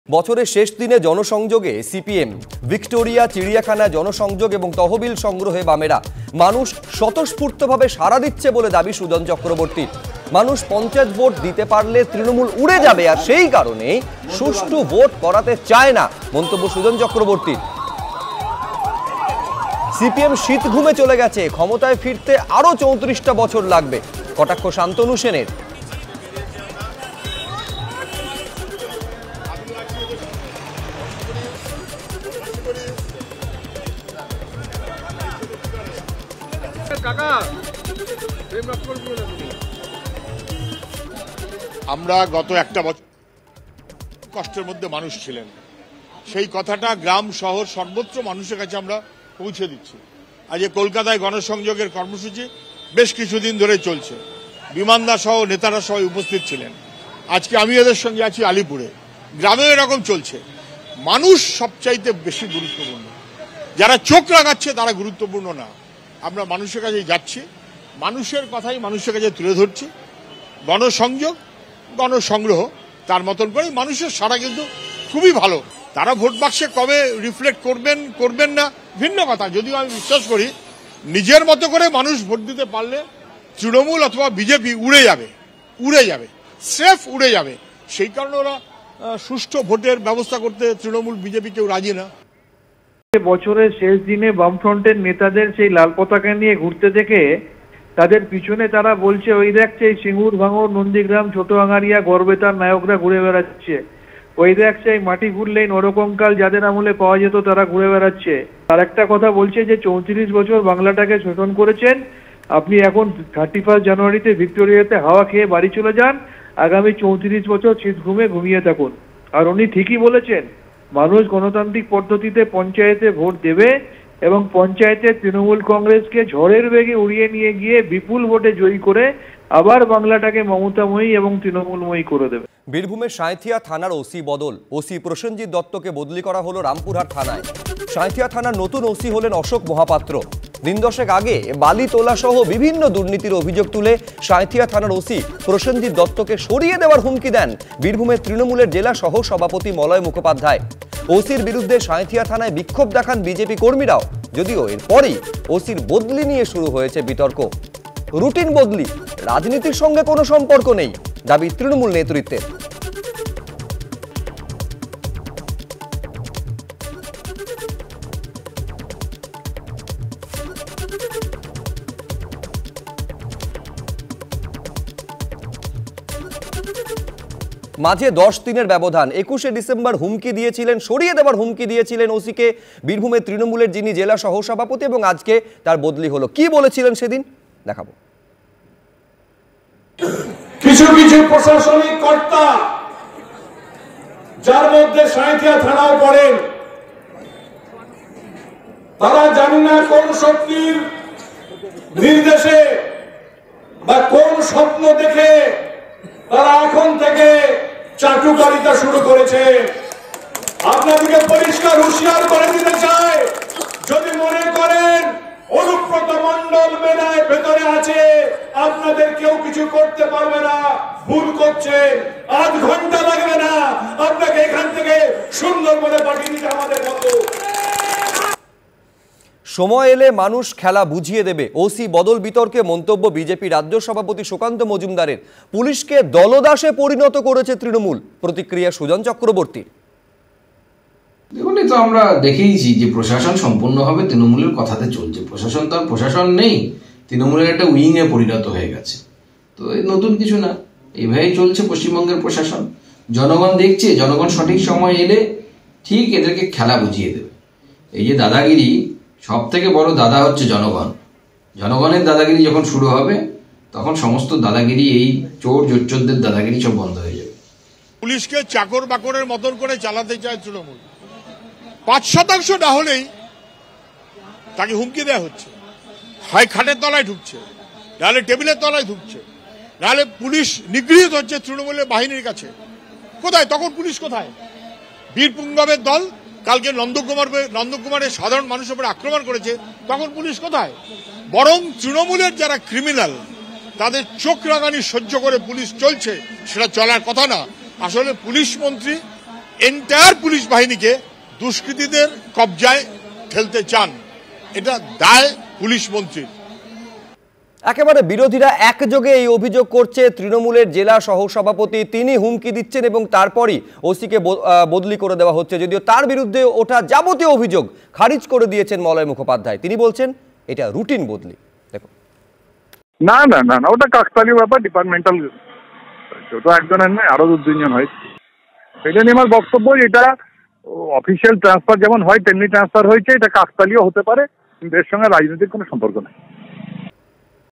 CPIM: 'মানুষ পঞ্চায়েতে ভোট দিতে পারলে তৃণমূল উড়ে যাবে', বছরের শেষদিনে জনসংযোগে সিপিএম
বছরের শেষদিনে জনসংযোগে সিপিএম। ভিক্টোরিয়া, চিড়িয়াখানায় জনসংযোগ ও তহবিল সংগ্রহে বামেরা। মানুষ স্বতঃস্ফূর্ত ভাবে সাড়া দিচ্ছে বলে দাবি সুজন চক্রবর্তীর। 'মানুষ পঞ্চায়েতে ভোট দিতে পারলে তৃণমূল উড়ে যাবে', সেই কারণেই সুষ্ঠু ভোট করাতে চায় না, মন্তব্য সুজন চক্রবর্তীর।